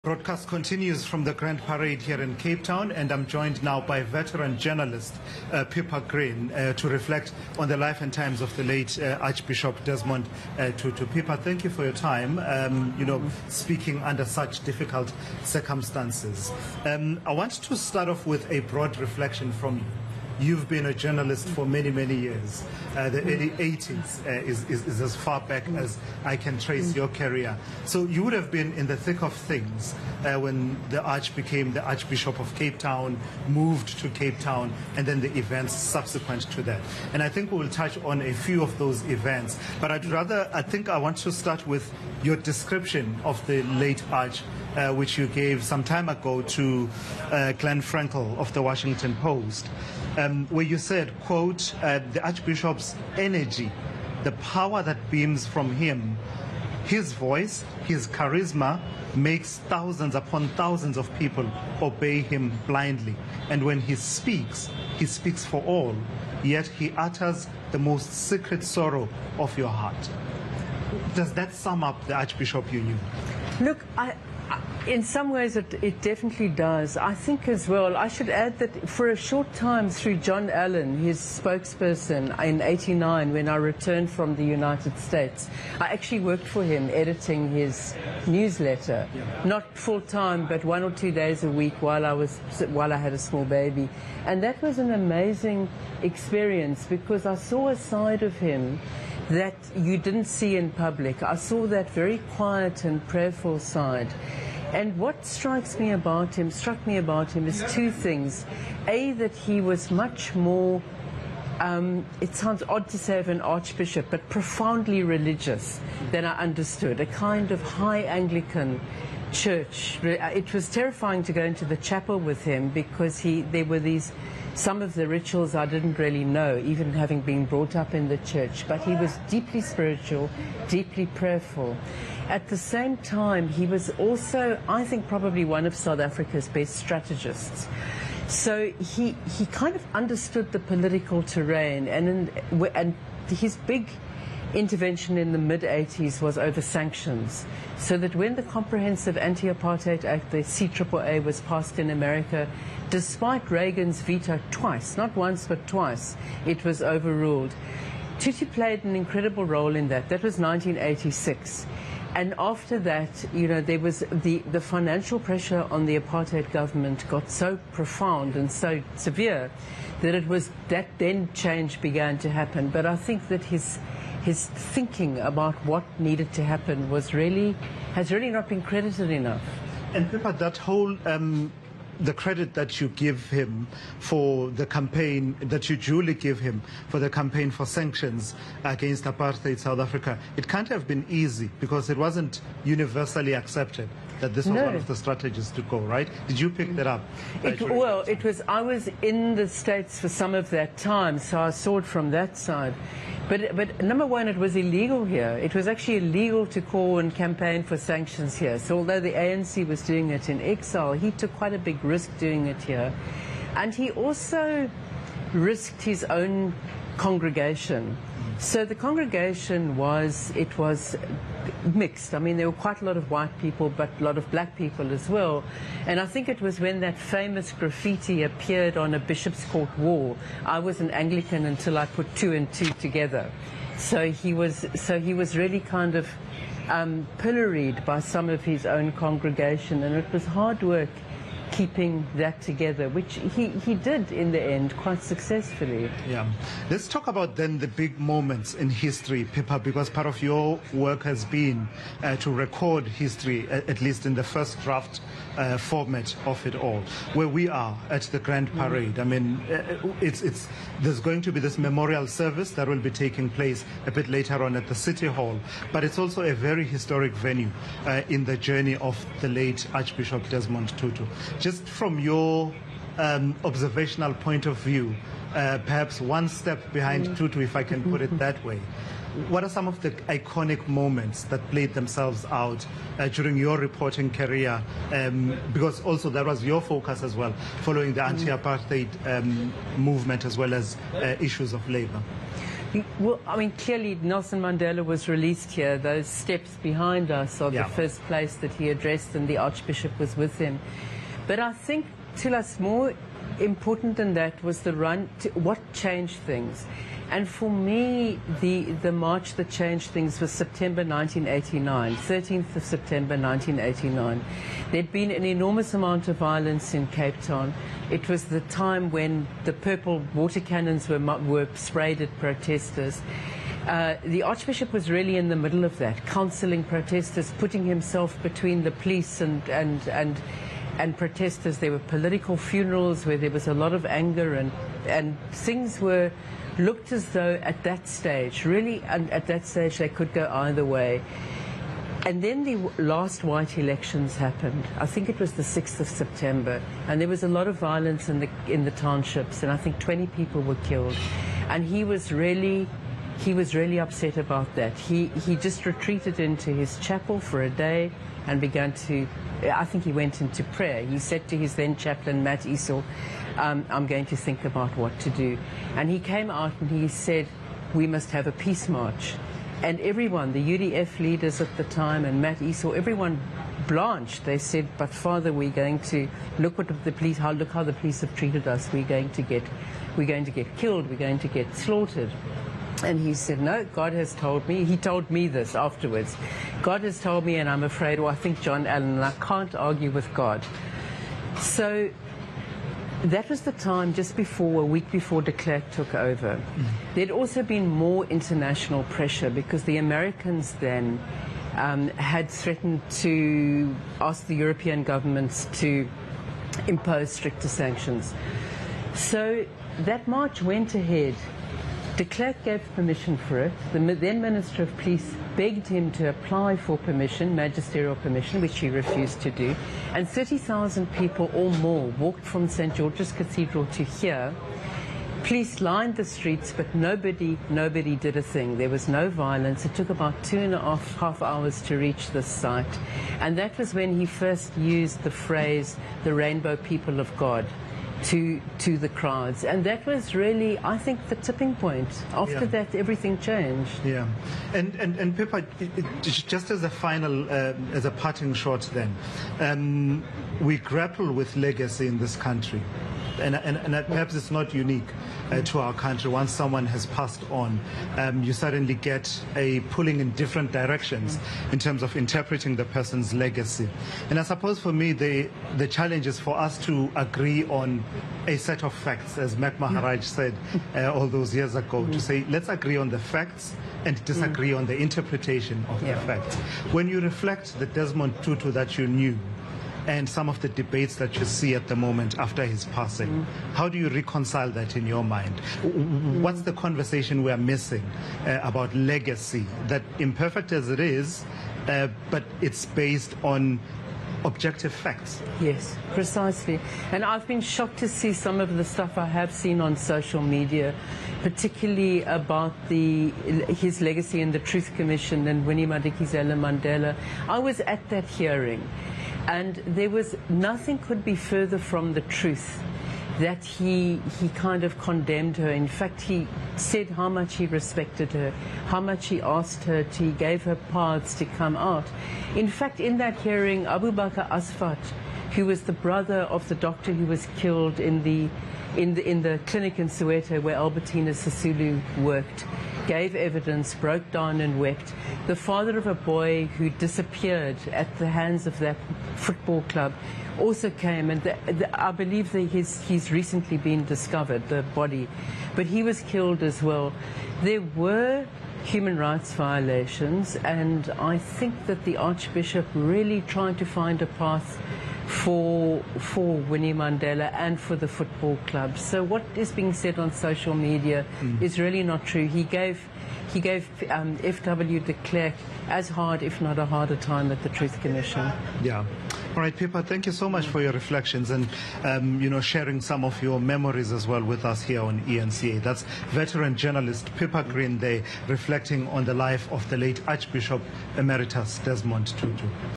Reflecting on the life and legacy of Desmond Tutu
eNCA's Thulasizwe Simelane spoke with Journalist and Writer, Pippa Green on the life and legacy of Archbishop Desmond Tutu. The City of Cape Town is honouring Tutu with an interfaith service. Courtesy